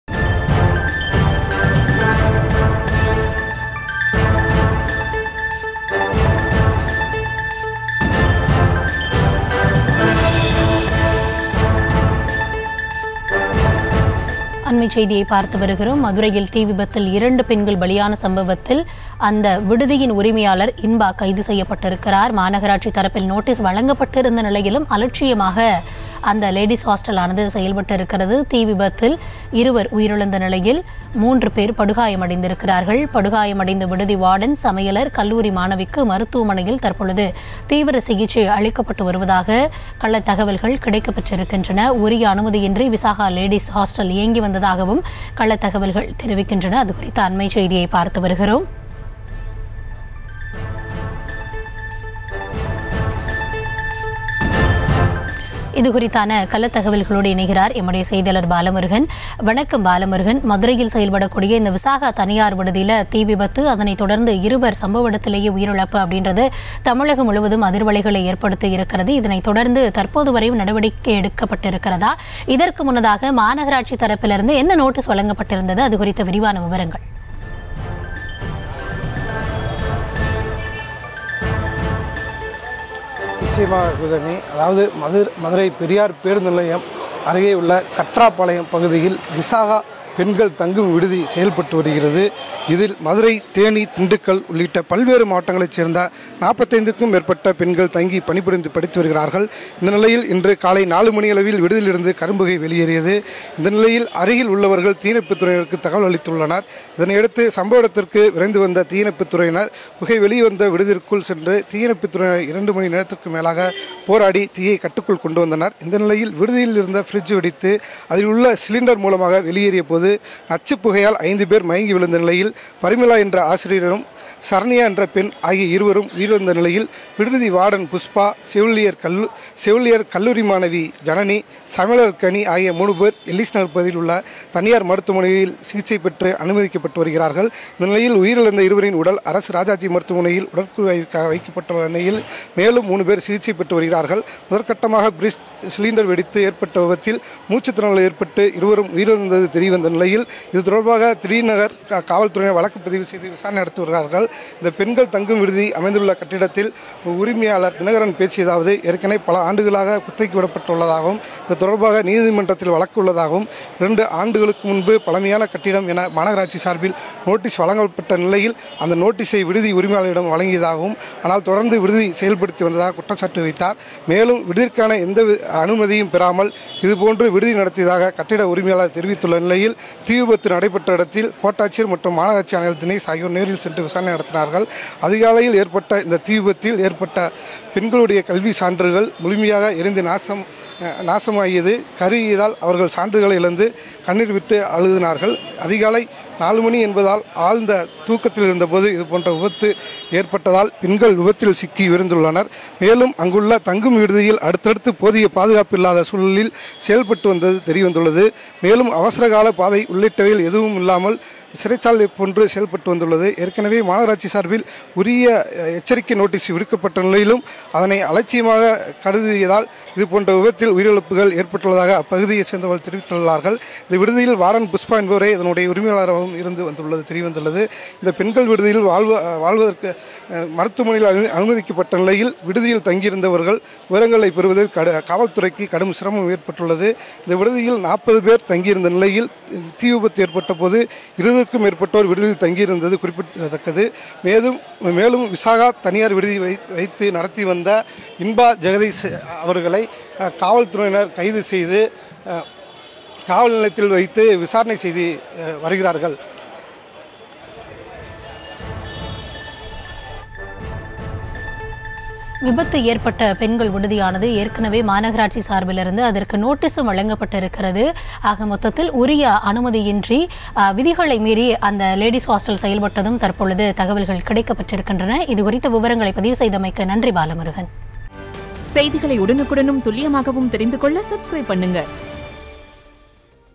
சிறைச்சாலை போன்று செயல்பட்டு வந்த விடுதி.. Hostel உரிமையாளர் கைது.. இரு உயிர்கள் பறிபோன சோக சம்பவம்
சிறைச்சாலை போன்று செயல்பட்டு வந்த விடுதி.. ஹாஸ்டல் உரிமையாளர் கைது.. இரு உயிர்கள் பறிபோன சோக சம்பவம்..